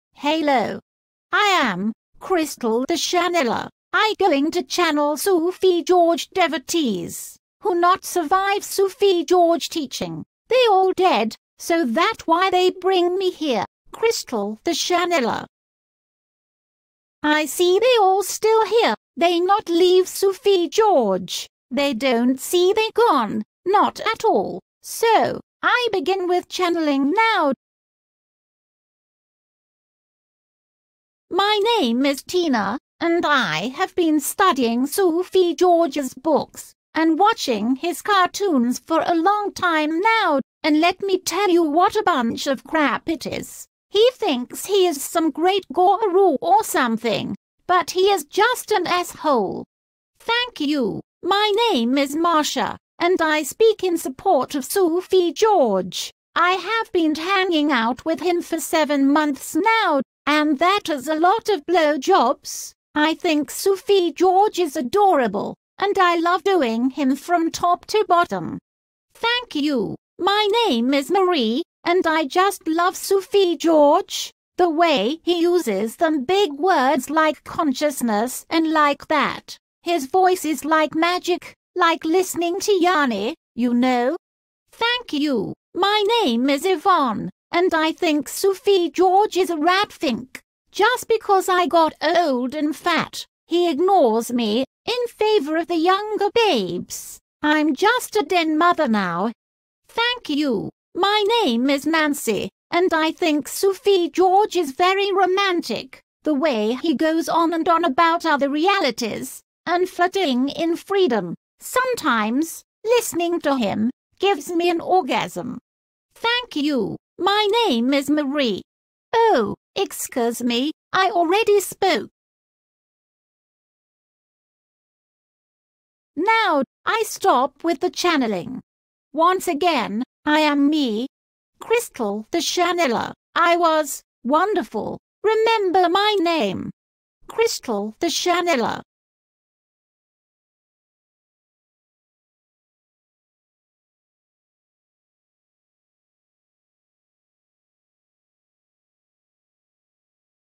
Sufi George hears testimonials from departed degenerates.
Crystal the channeler channels some babes who didn't survive the Sufi George teachings.